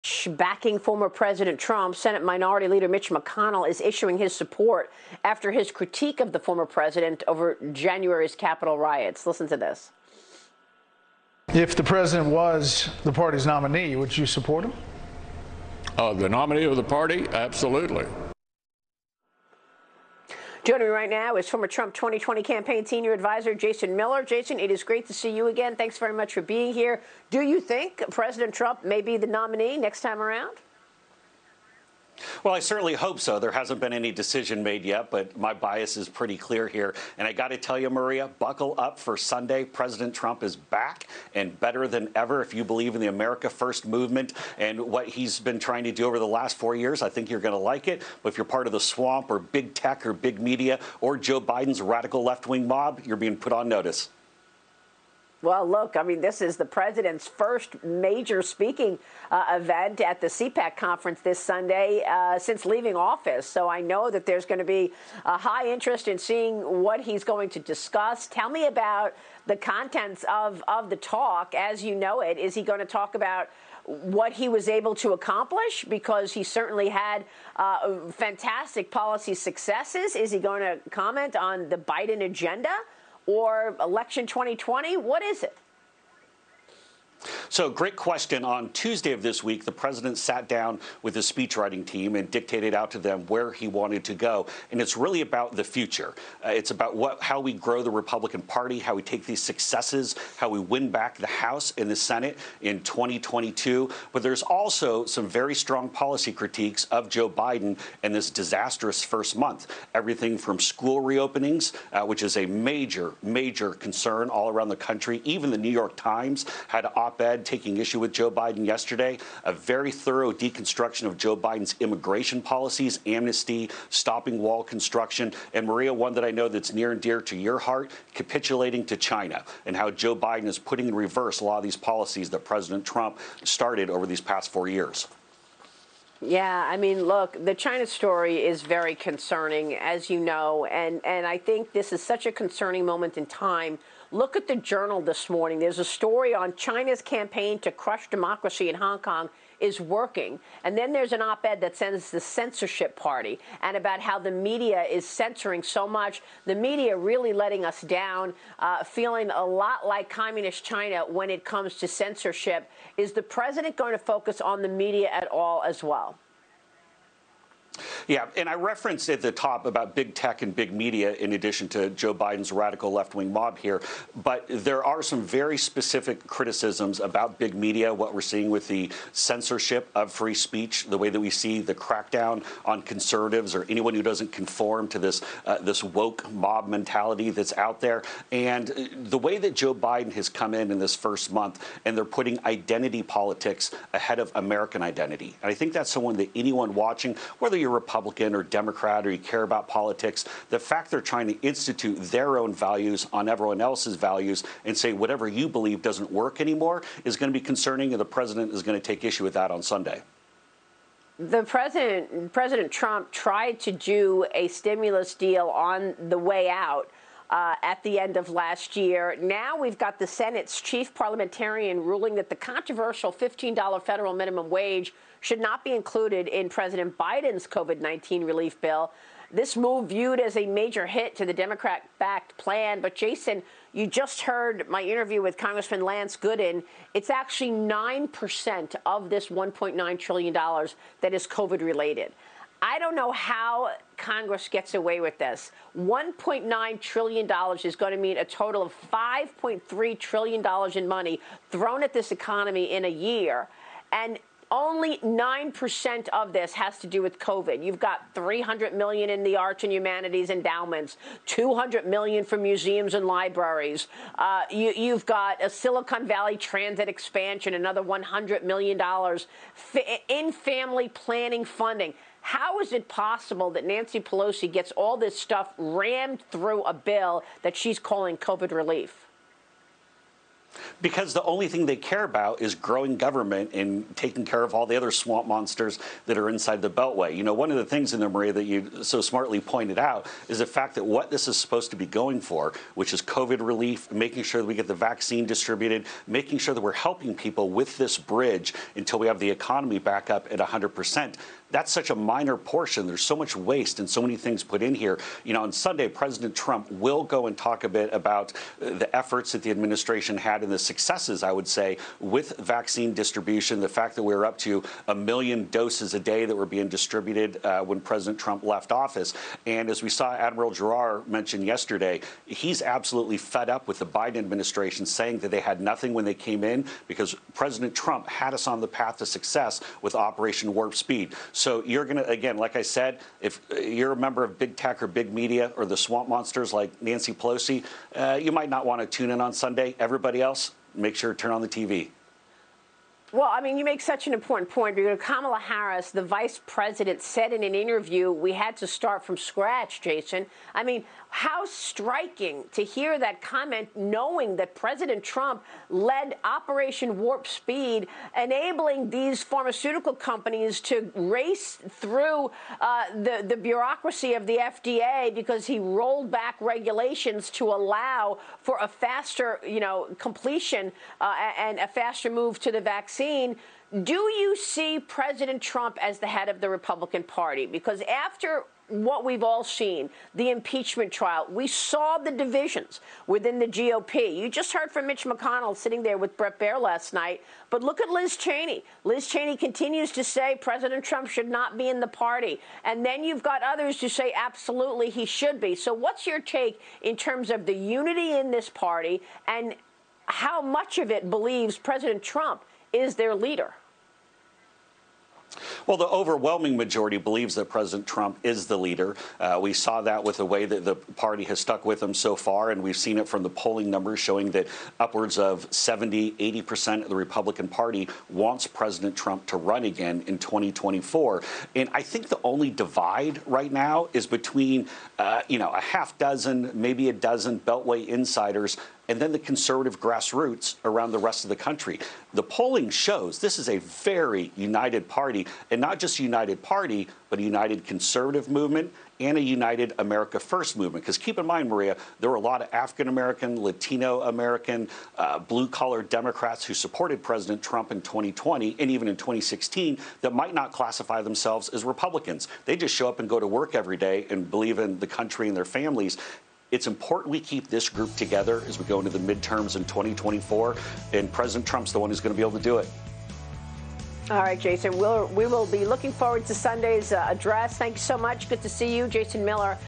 'Buckle up' for CPAC, Trump is back and better than ever: Jason Miller
Jason Miller, former Trump 2020 campaign senior adviser, previews the former president's first speech after leaving office at CPAC and addresses a potential 2024 run.

Follow Fox Business on Facebook Follow Fox Business on Twitter Follow Fox Business on Instagram